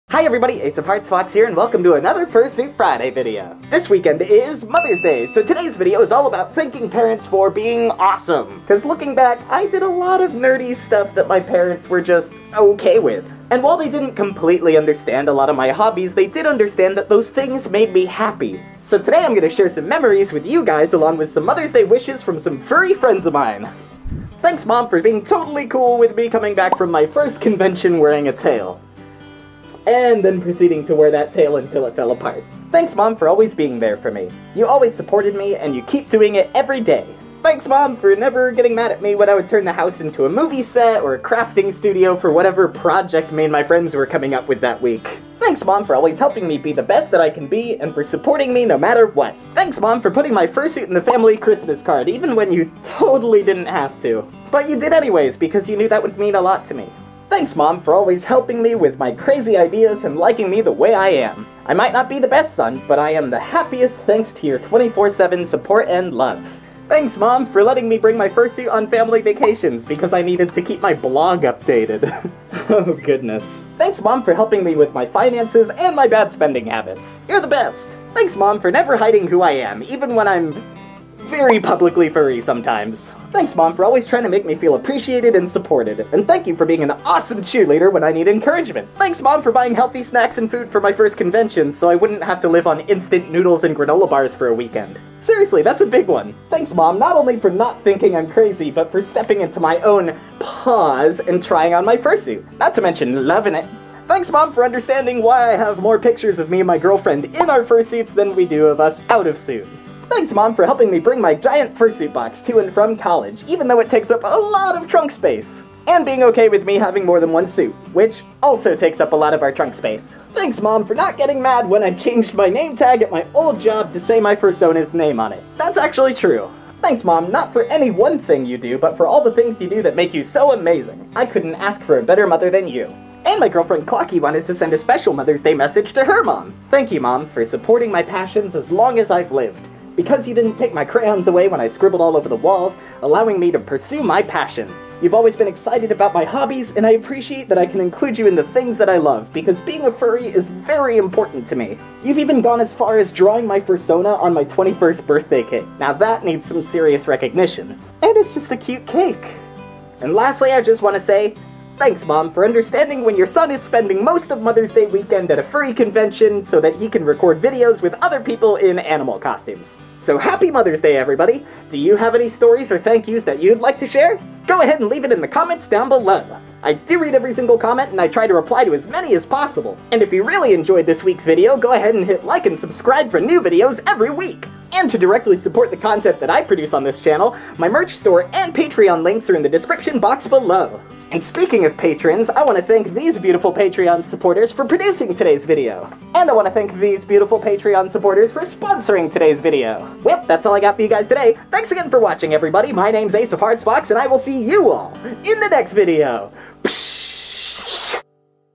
Thanks, Mom!
Happy Fursuit Friday, and Happy Mother's Day, everybody! Being a parent is pretty tough, and it's even more of a challenge, when your kids have difficult to understand hobbies! So today, some furry friends, and I are going to share some memories and Mother's Day wishes!